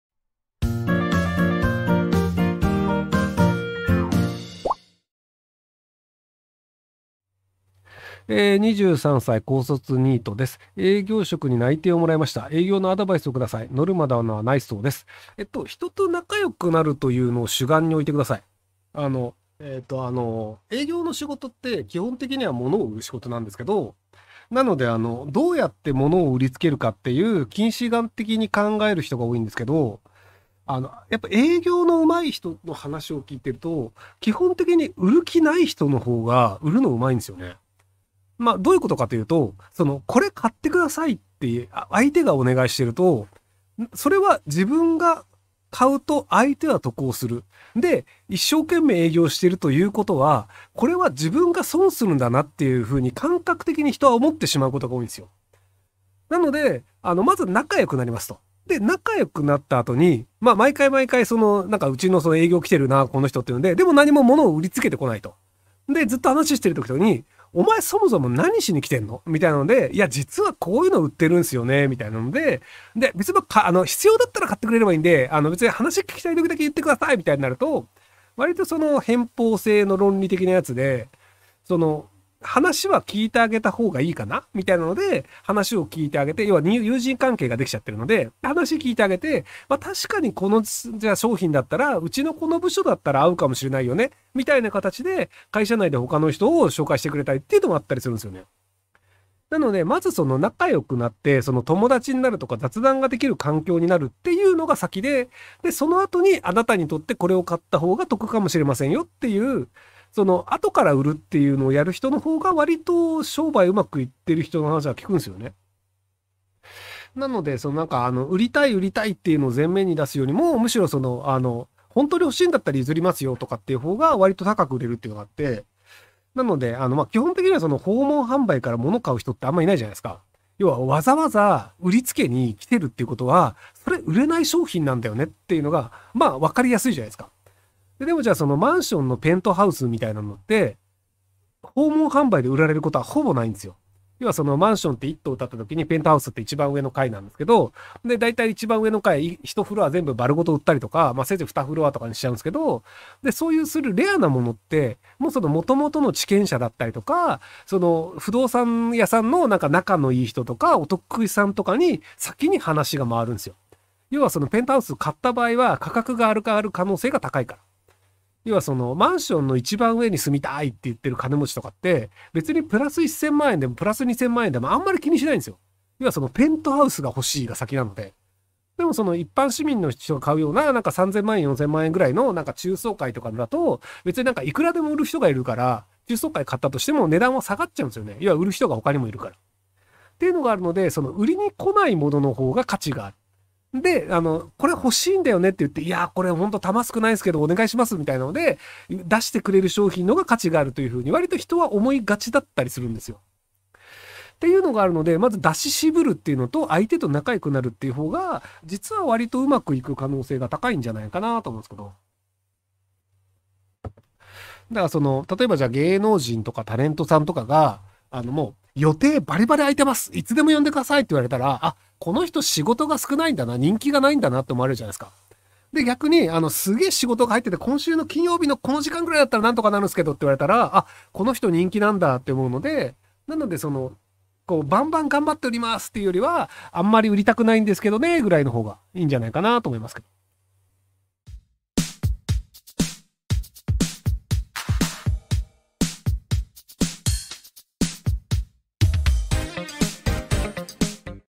【新社会人必見】"優秀な"営業マンこそモノを売りません。営業マンがまずやるべきことは〇〇をすることです！【ひろゆき切り抜き】
毎日19：00にひろゆき切り抜き動画を投稿します。